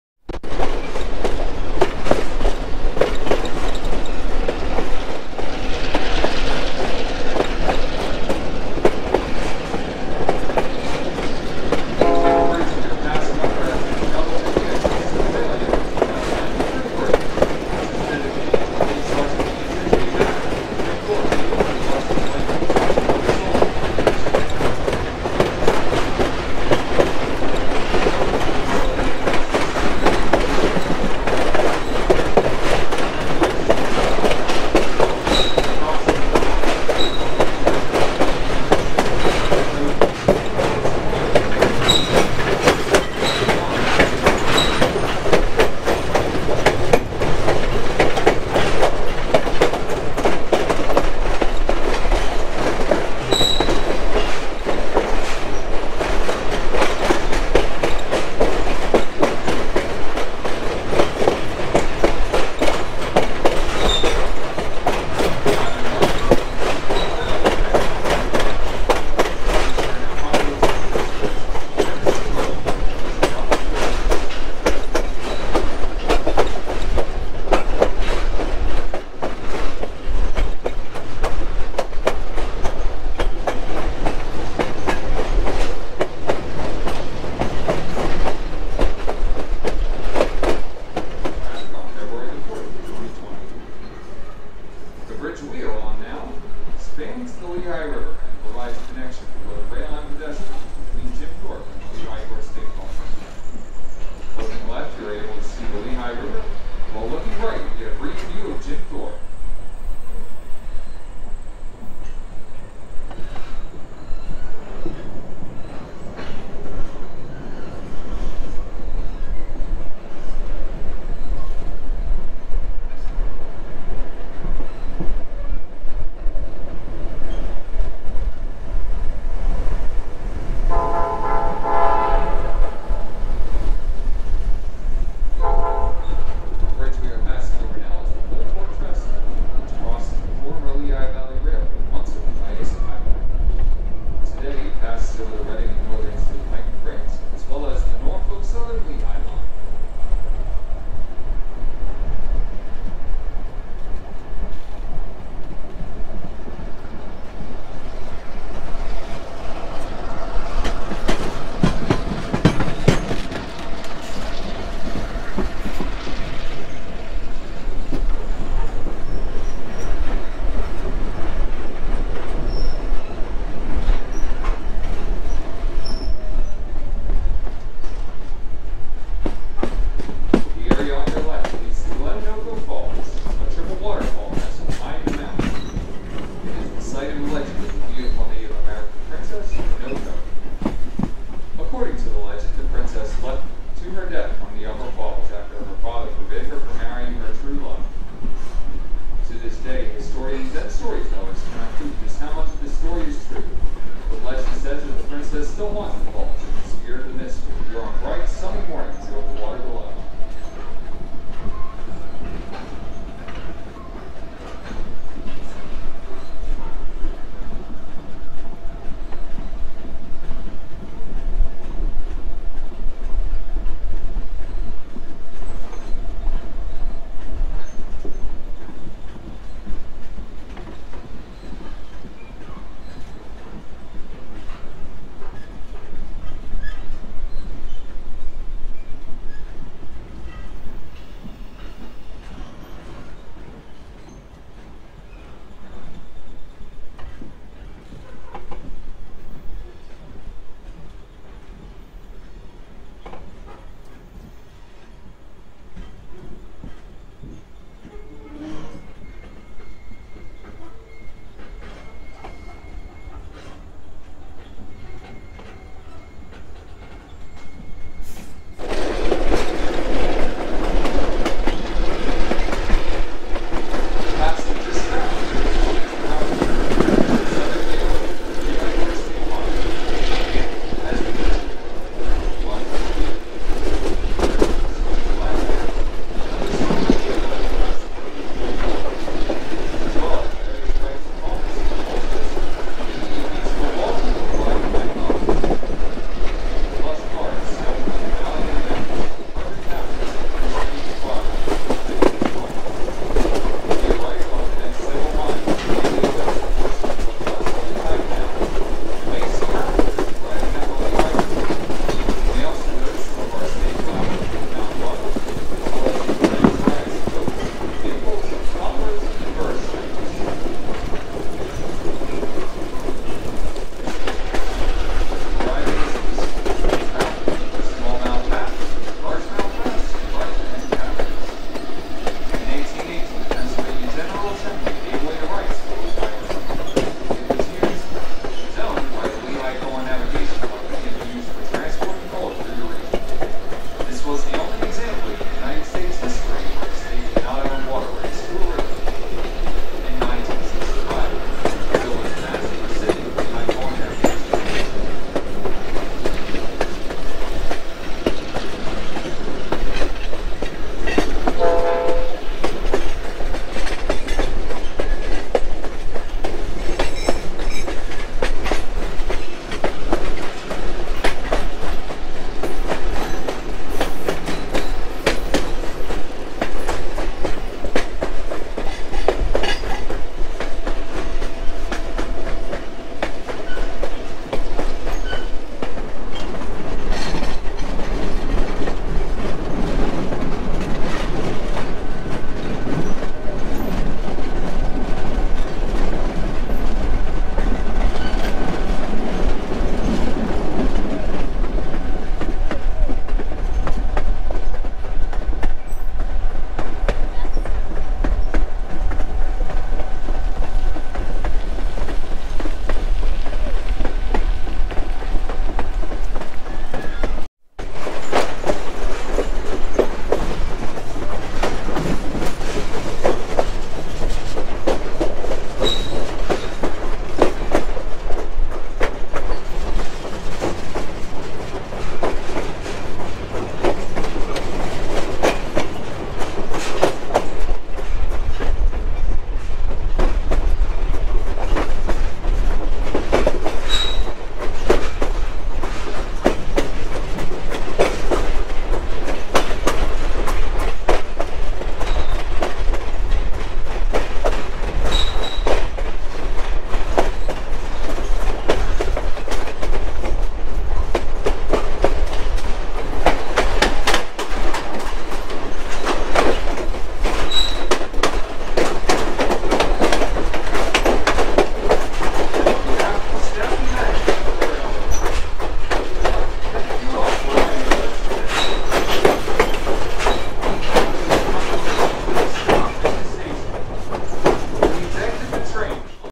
Lehigh Gorge Scenic Railway Train Ride
Take a ride on the train at Jim Thorpe!